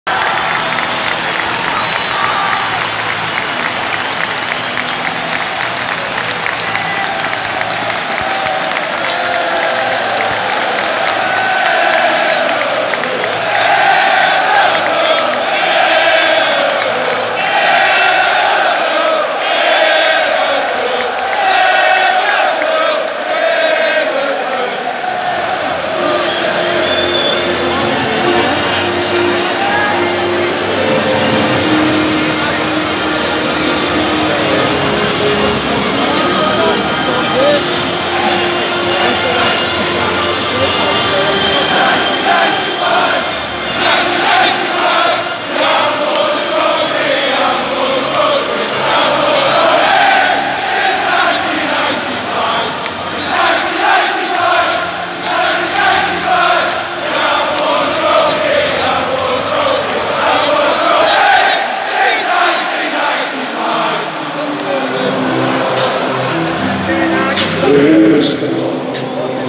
Since 1995 (watch till end if you don't get the Title name)🤣👍
Awesome night at anfield it was also flag day and as you can see it was amazing. Liverpool ran out winners final score was liverpool 2 everton 0 another derby win for the reds.